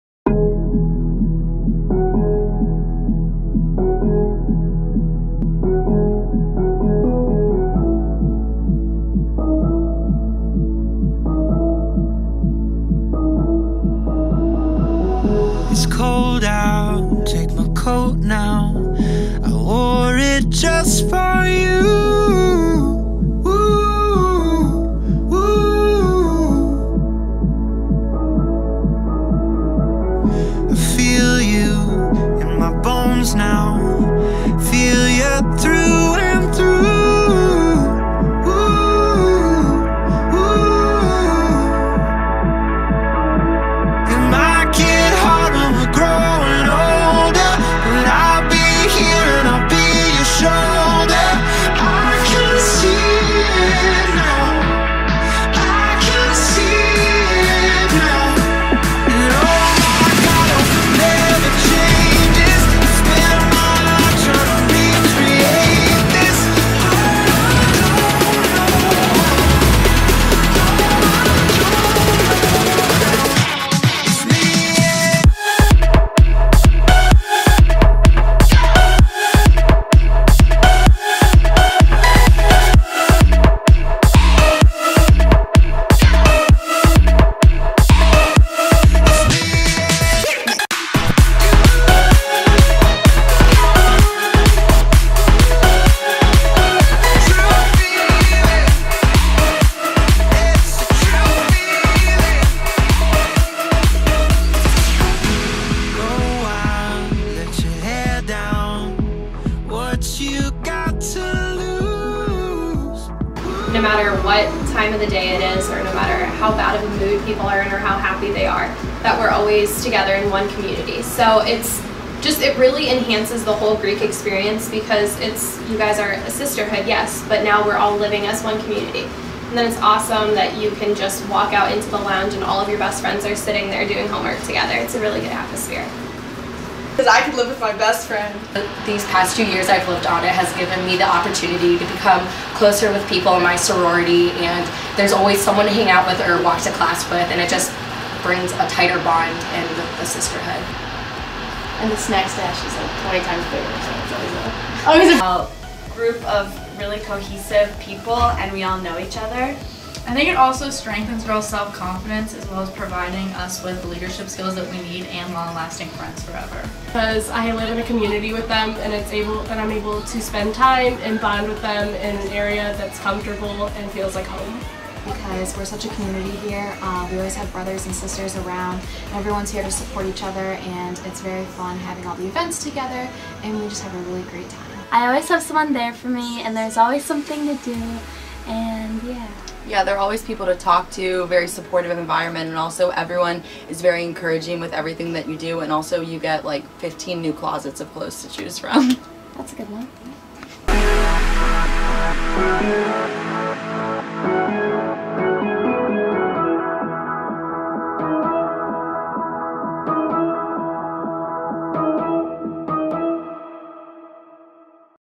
John Carroll University Sorority Floor Tour Video 2018
An introduction to Hamlin Hall for Sorority Formal Recruitment 2018!